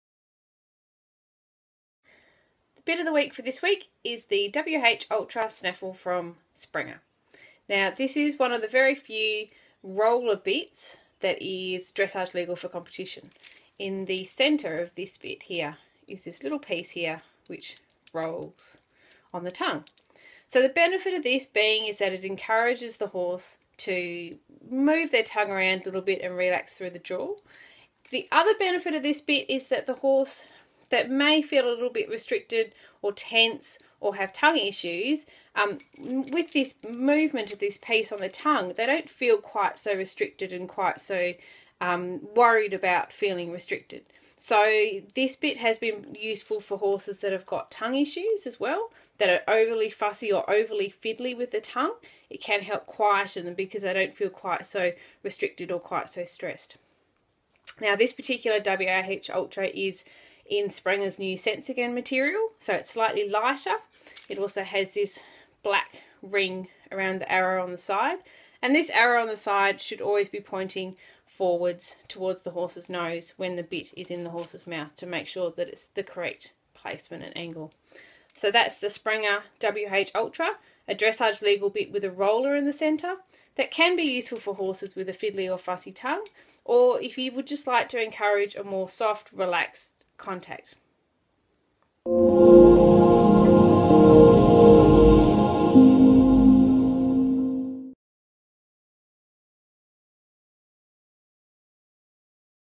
Bit of the Week - WH Ultra Snaffle
The WH Ultra Snaffle- a dressage legal bit with a central roller that can be helpful with horses that feel restricted or are are fussy in the tongue.